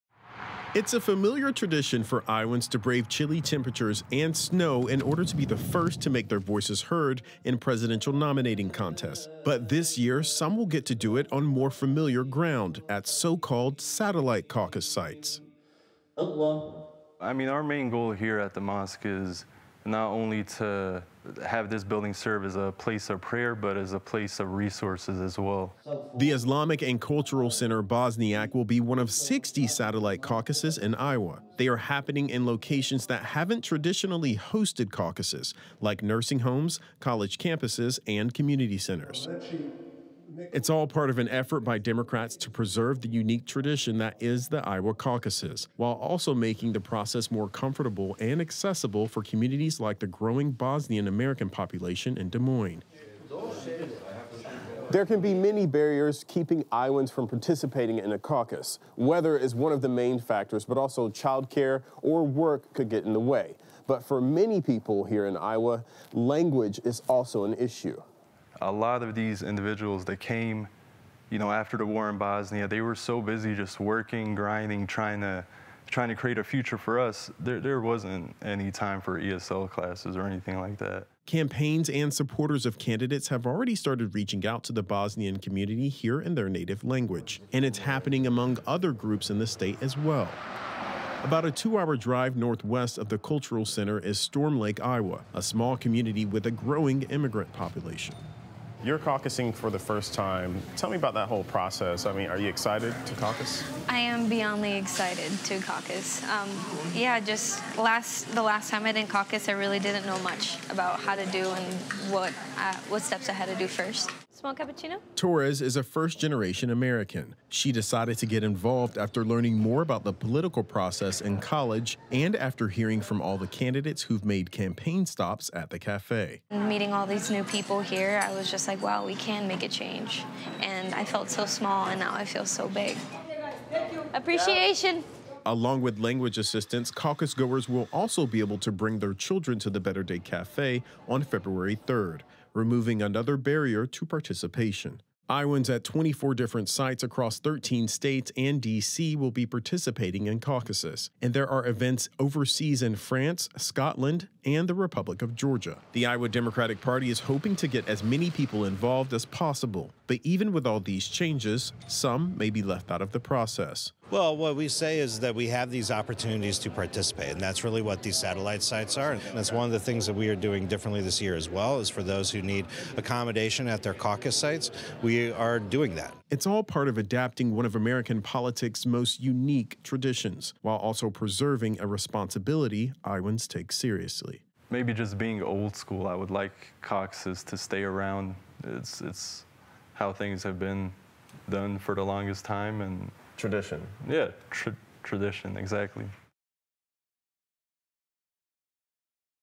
Iowa Democrats Aim To Increase Turnout With 'Satellite' Caucuses
In an effort to increase voter participation, Iowa Democrats will host 87 'satellite' caucus locations within and outside the state.